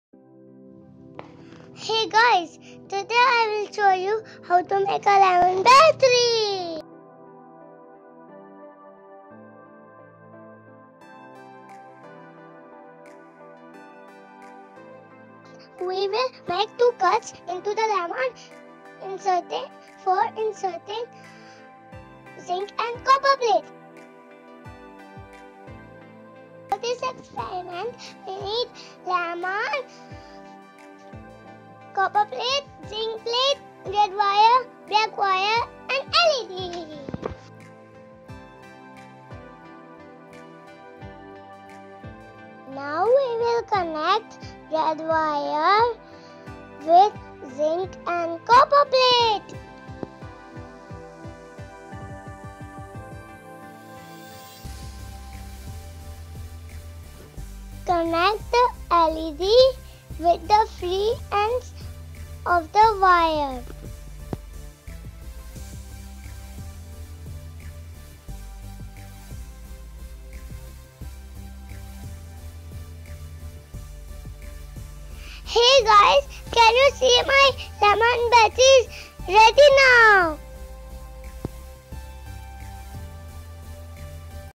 how to make a Lemon battery | DIY Lemon Battery LED Light up | Experiment for kids
DIY : how to generate electricity using lemon at home with the help of copper and zinc plate at home, how to make lemon battery at home